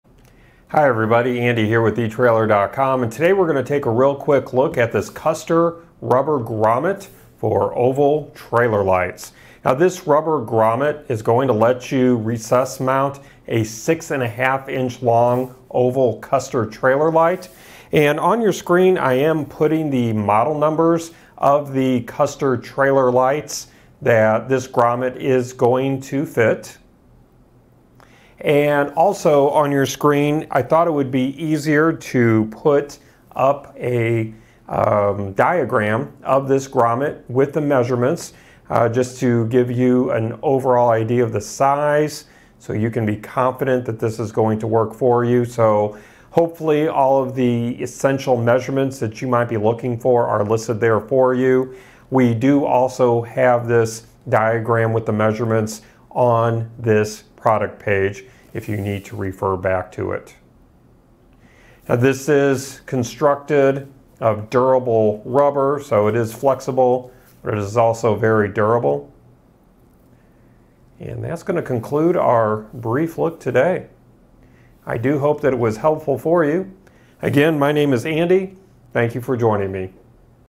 etrailer | Comprehensive Review: Custer Rubber Grommet for 6-1/2" Long Oval Trailer Lights
Hi everybody, Andy here with etrailer.com.  And today we're gonna take a real quick look at this Custer Rubber Grommet for oval trailer lights.  Now this Rubber Grommet is going to let you recess mount a six and a half inch long oval Custer trailer light.  And on your screen I am putting the model numbers of the Custer trailer lights that this Grommet is going to fit.  And also on your screen, I thought it would be easier to put up a diagram of this grommet with the measurements just to give you an overall idea of the size, so you can be confident that this is going to work for you.  So hopefully all of the essential measurements that you might be looking for are listed there for you.

We do also have this diagram with the measurements on this product page if you need to refer back to it.  Now this is constructed of durable rubber, so it is flexible, but it is also very durable.  And that's gonna conclude our brief look today.  I do hope that it was helpful for you.  Again, my name is Andy.